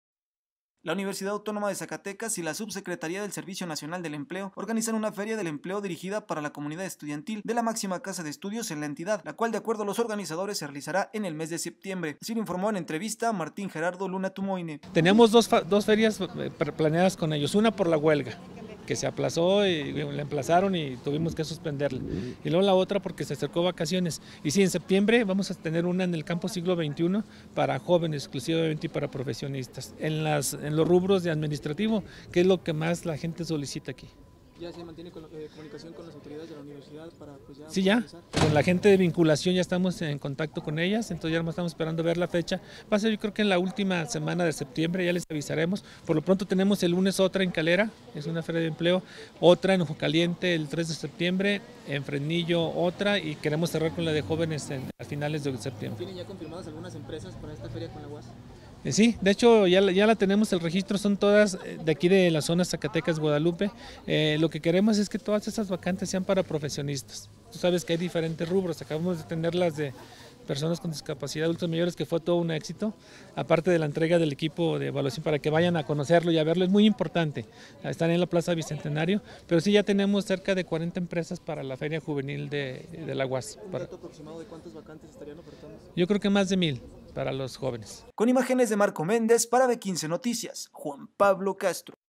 REALIZARÁN FERIA DEL EMPLEO EXCLUSIVA PARA COMUNIDAD ESTUDIANTIL DE LA UAZ
La Universidad Autónoma de Zacatecas y la subsecretaria del Servicio Nacional del Empleo organizan una feria del empelo dirigida para la comunidad estudiantil de la máxima casa de estudios en la entidad, la cual de acuerdo  a los organizadores se realizará en el mes de septiembre presentando una oferta laboral de más de mil vacantes, al respecto habla Martin Luna Tumoine responsable de esta subsecretaria.